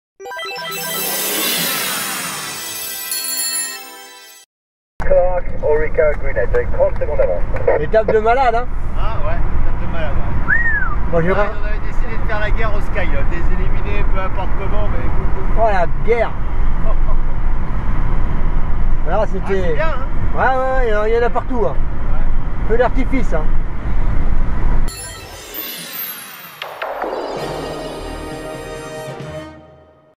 Tour de France 2013 - L'image du jour #9 : "Une étape de malades"
Discussion sur le pouce entre Franck Pineau, directeur sportif de l'équipe FDJ.fr, et Christian Guiberteau, son homologue chez Argos-Shimano. Tous les deux d'accord : "Y en a partout".